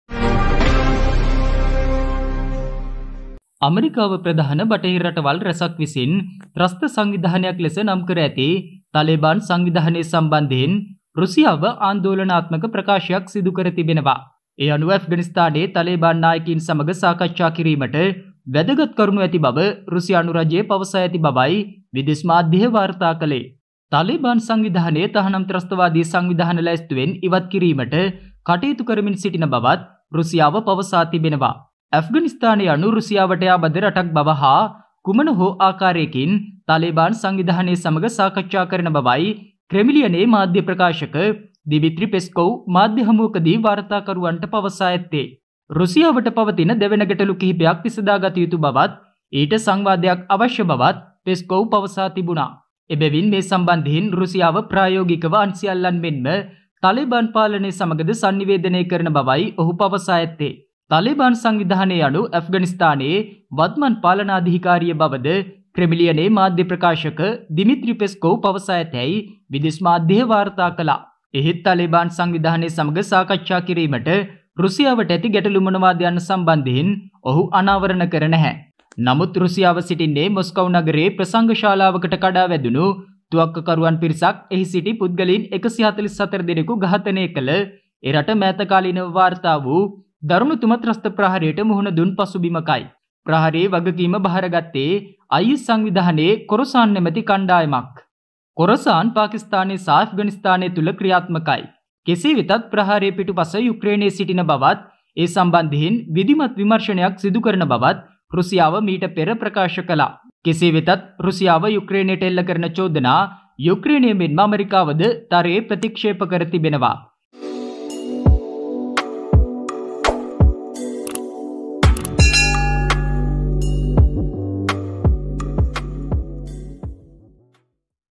තලේබාන්ලා රකින්න රුසියාව පෙරට | Russia Says Working On Removing Taliban From Its Terrorist List
Russia said on Tuesday it had important matters to discuss with Afghanistan's Taliban leaders and was working to remove the Taliban from its list of banned terrorist organisations.
"This is a country that is next to us, and one way or another we communicate with them," Kremlin spokesman Dmitry Peskov told reporters.

"We need to resolve pressing issues, this also requires dialogue, so in this regard we communicate with them like practically everyone else - they are the de facto authority in Afghanistan."

Peskov did not elaborate on the "pressing issues", but Russia suffered its deadliest attack for 20 years last month when gunmen rushed into a concert hall outside Moscow, killing at least 144 people.

Islamic State terrorists claimed responsbility and US officials said they had intelligence that it was the network's Afghan branch, Islamic State Khorasan, that was responsible. Russia has said it is also investigating a Ukrainian link, something Kyiv and the United States have strongly rejected.